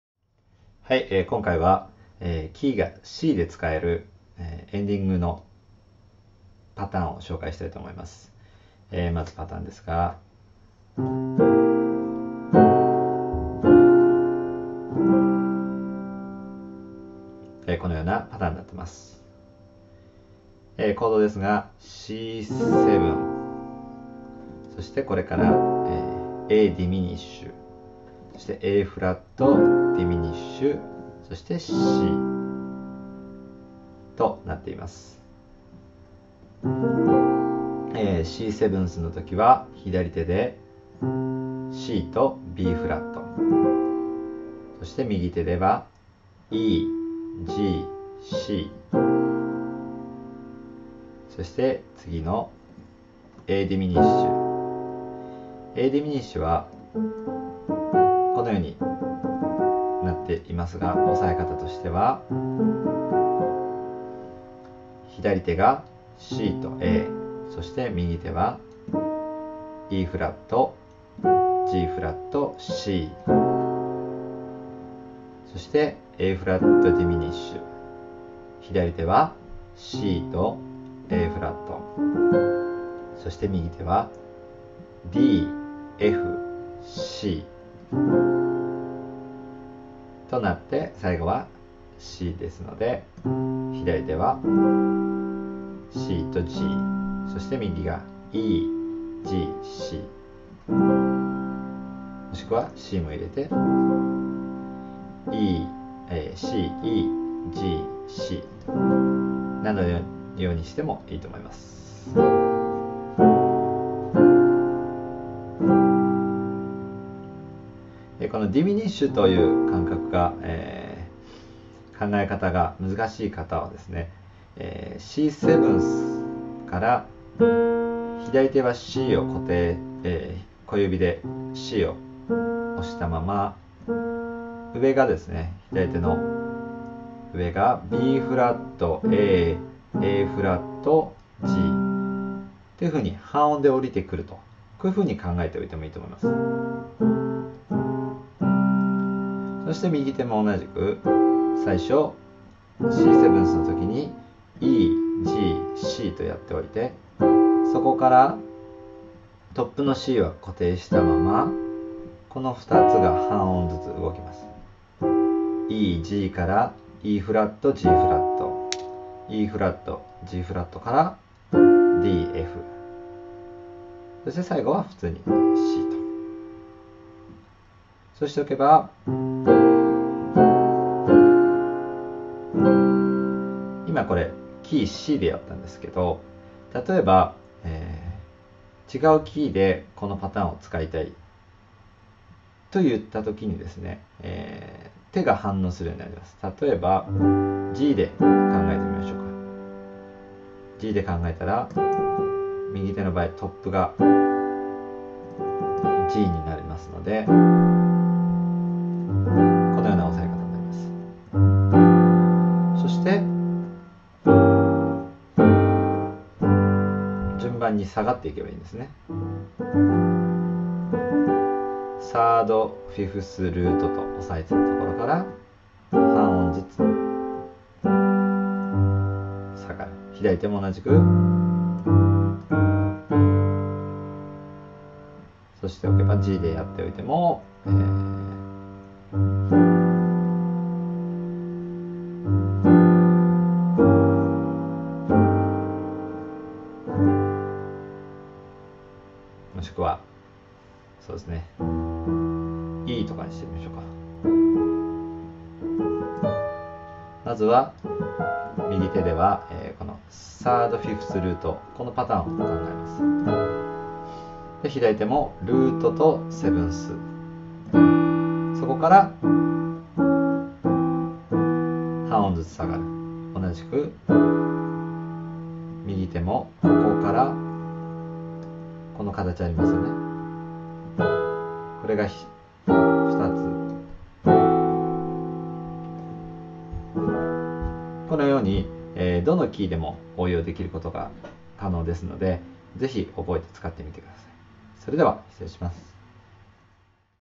ピアノ弾き語りエンディングフレーズ
Key=Cで使えるエンディングパターンです。Keyが変わっても使えるように、応用可能なパターンとして解説しています。